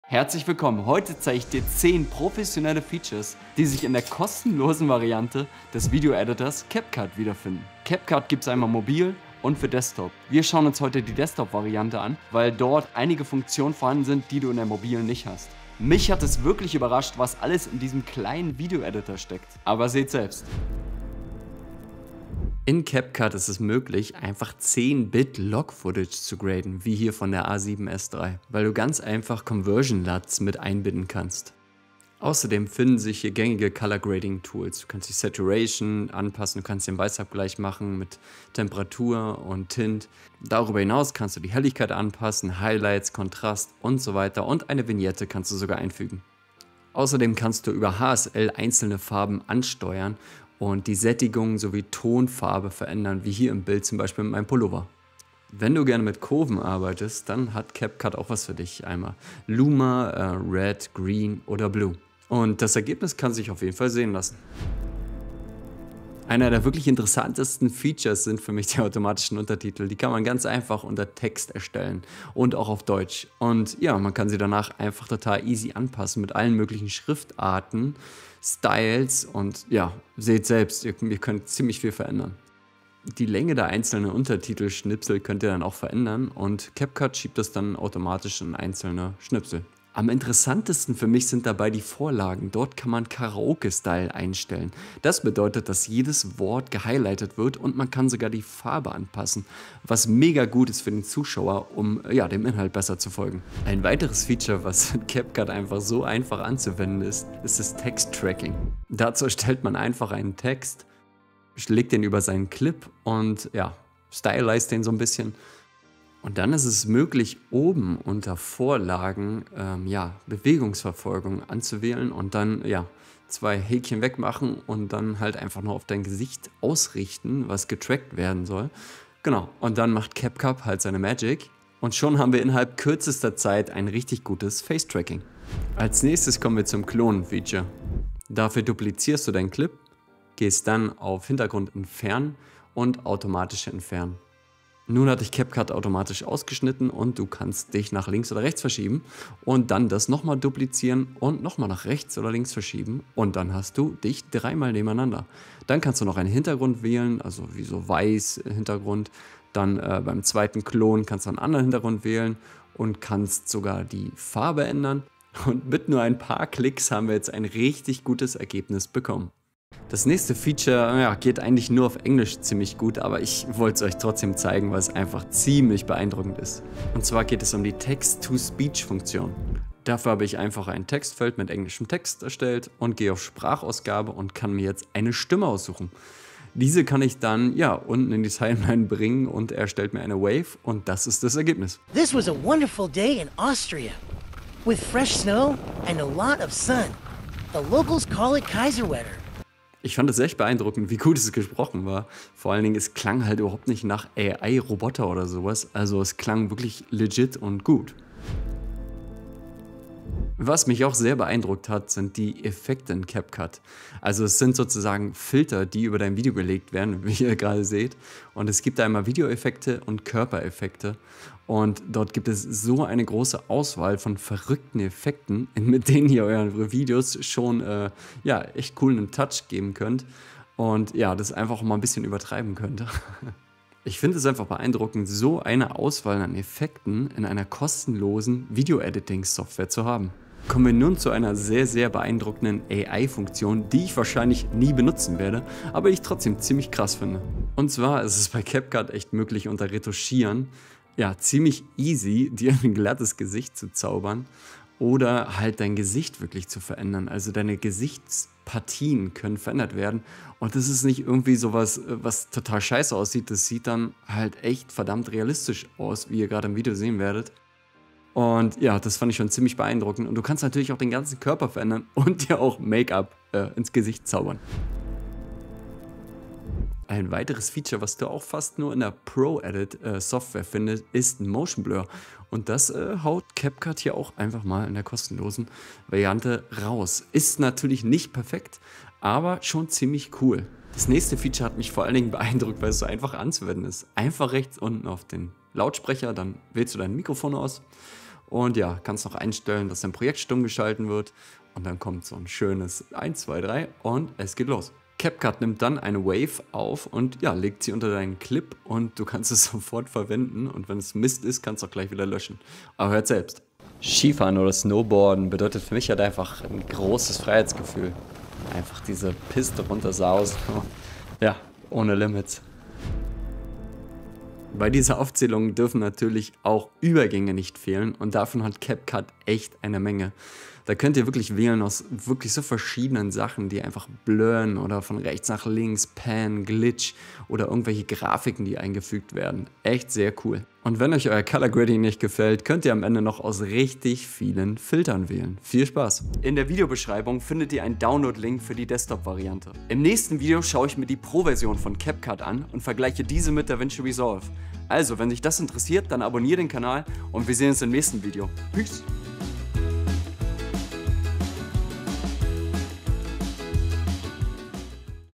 Top 10 Funktionen in CapCut (Kostenlos)
In diesem Video zeige ich dir 10 kostenlose professionelle Features, die sich in CapCut wiederfinden. Dabei bietet die Desktop Variante etwas mehr Effekte und Funktionen als in der mobilen Versionen. Die meisten Funktionen lassen sich aber auch in der mobilen Version von CapCut anwenden.

00:00 Intro
00:20 Color Grading Log
01:05 Automatische Untertitel
01:44 Text Tracking
02:16 Klonen Maskieren
02:50 Text Sprachausgabe
03:28 Effekte
04:04 Gesicht & Körper Anpassen AI
04:35 Motion Blur
04:51 Voice Over
05:23 Übergänge(Transitions)
05:46 Download